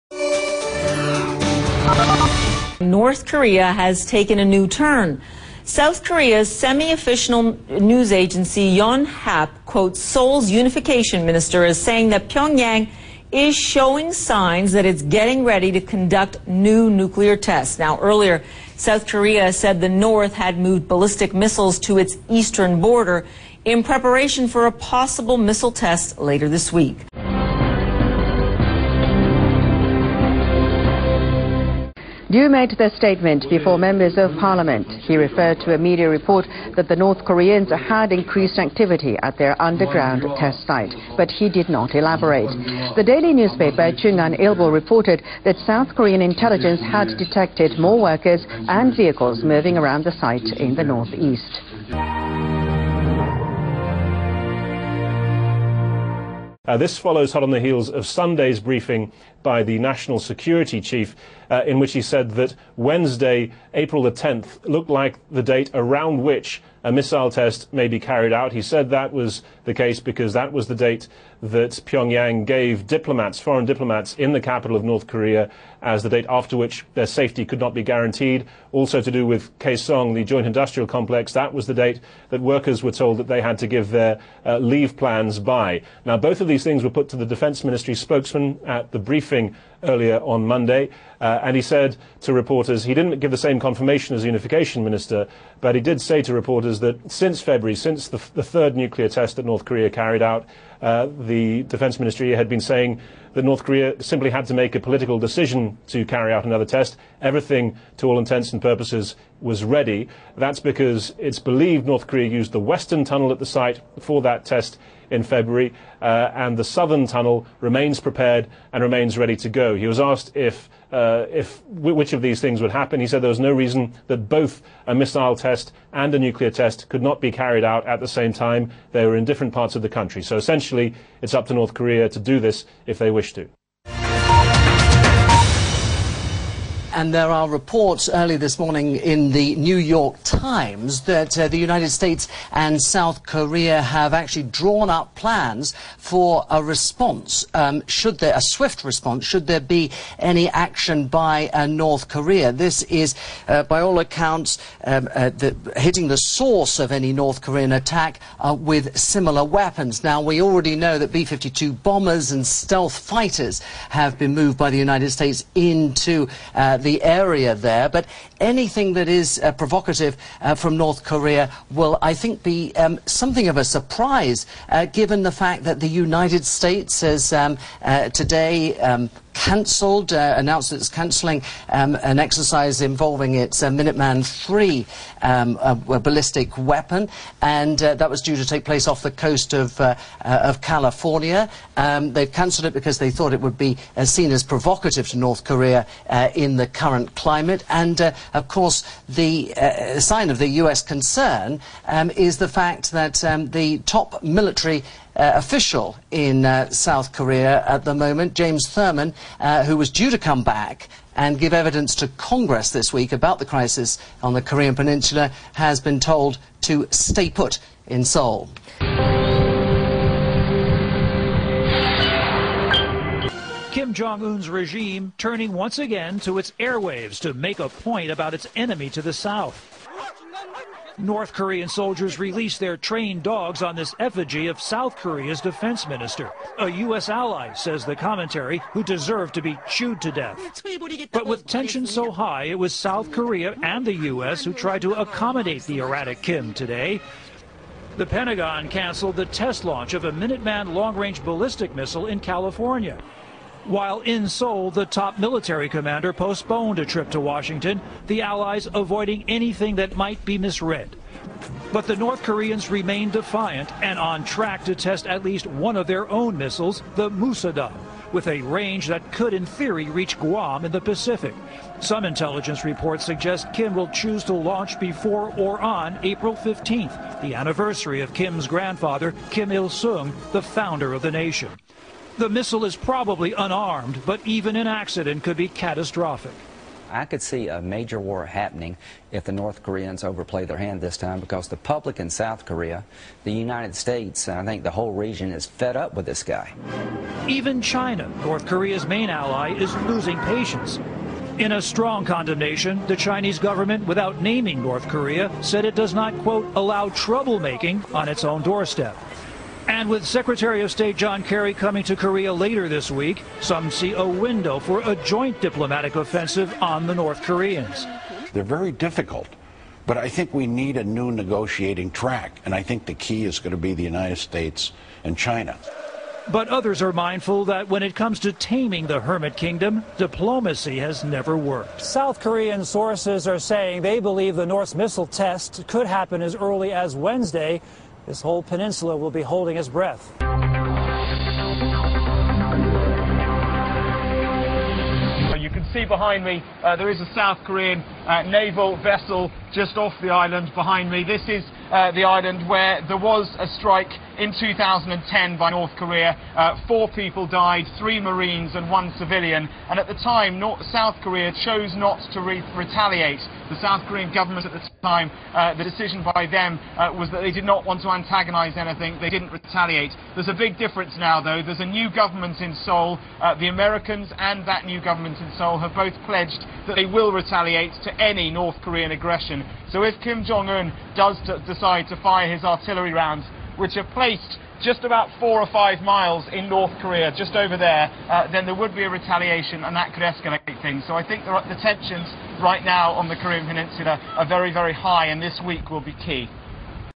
South Korea: 'Indication' North Korea prepping for nuke test
South Korea's point man on North Korea said on Monday there is an "indication" that Pyongyang is preparing for a fourth nuclear test, a day after another Seoul official said a Pyongyang missile test may be in the works.